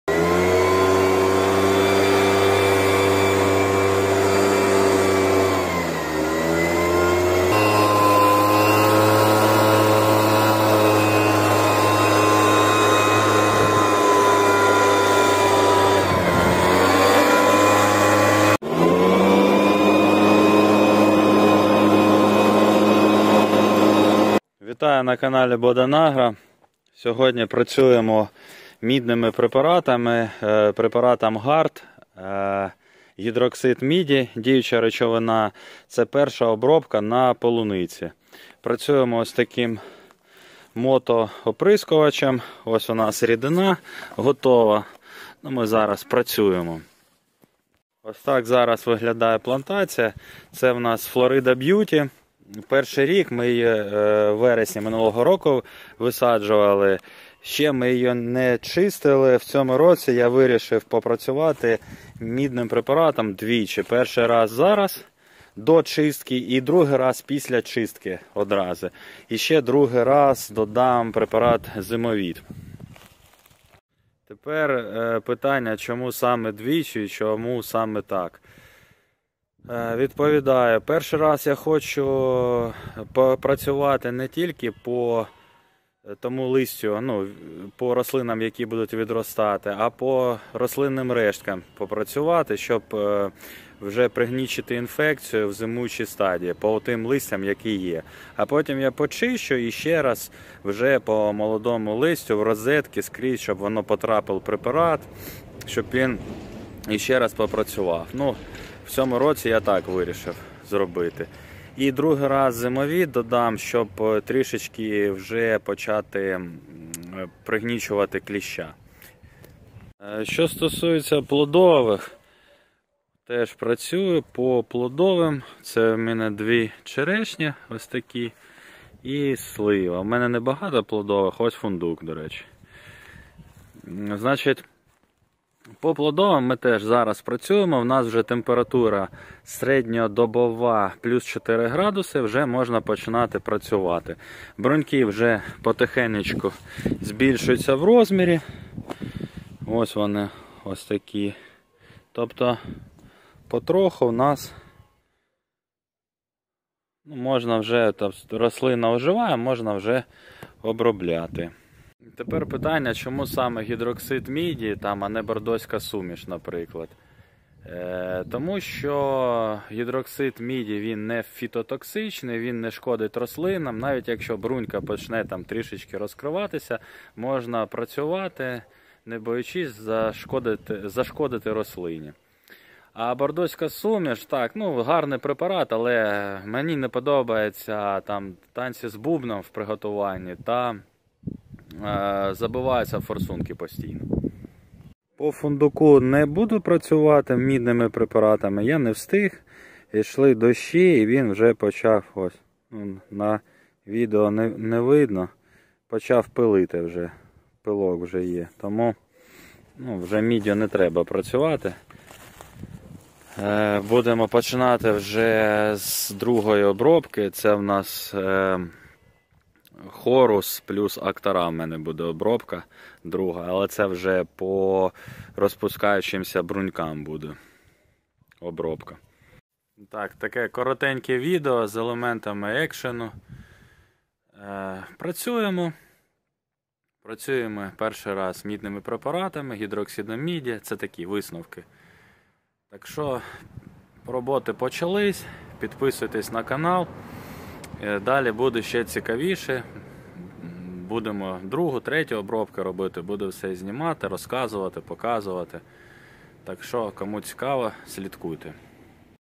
Перша обробка саду, полуниці навесні
Перша обробка саду, полуниці навесні. Працюємо гідроксидом міді. Препарат Гарт.
Bodan Agro